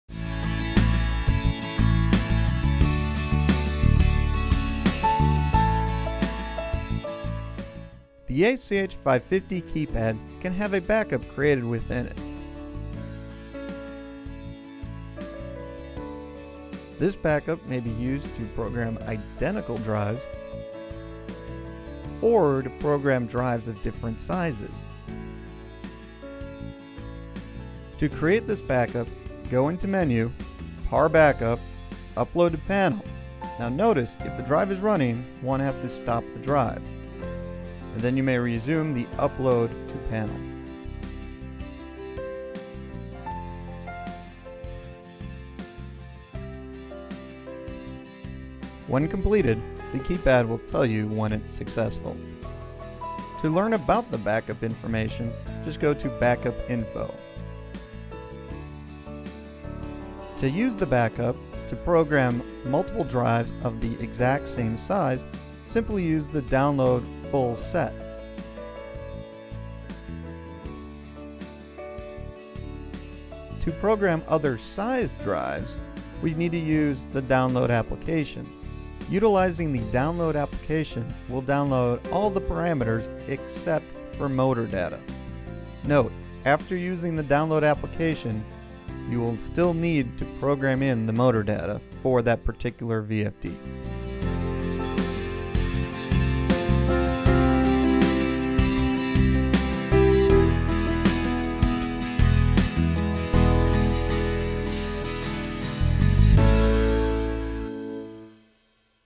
ACH550 Parameter Backup
This video shows how to save a backup of the parameters in the drive.  Also, this shows how to download from the backup to program drives as well.